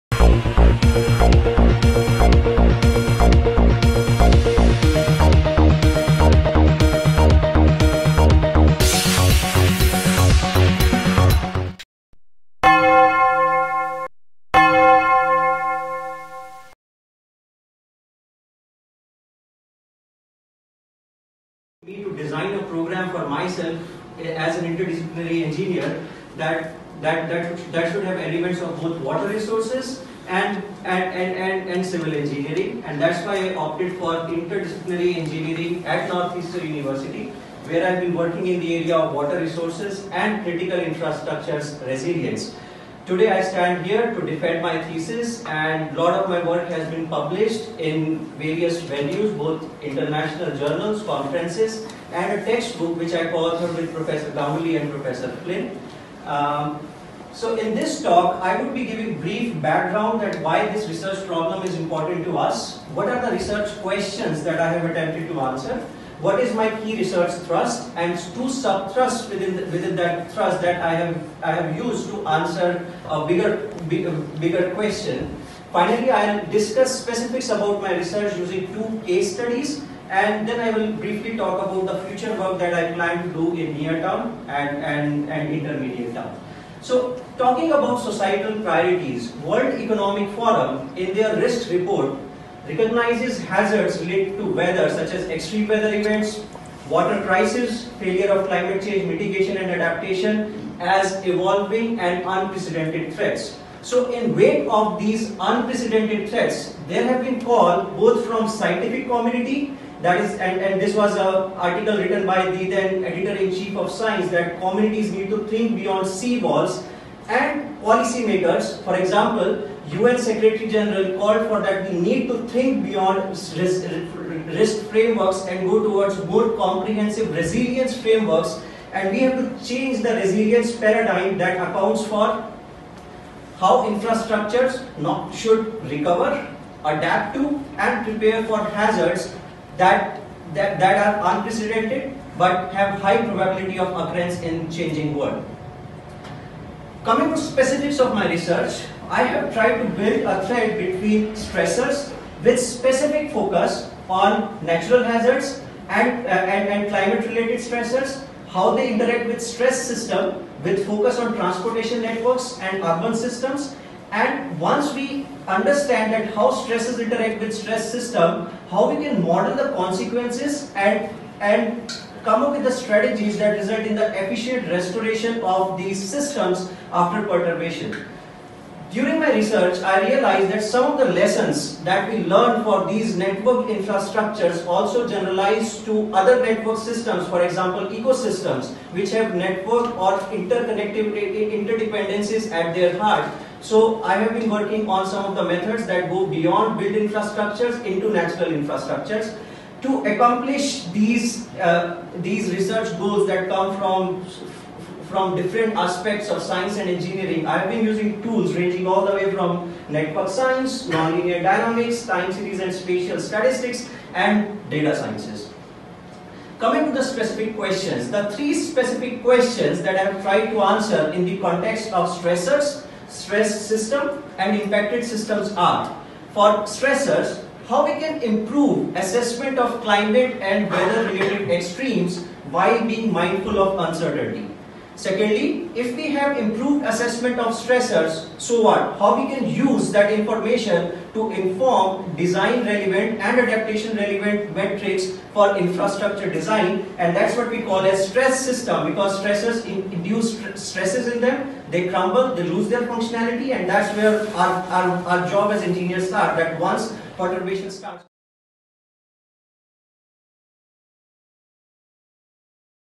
Dr Udit Bhatia presentation at Northeastern University Boston
Dr Udit Bhatia presentation to his professors at Northeastern University Boston.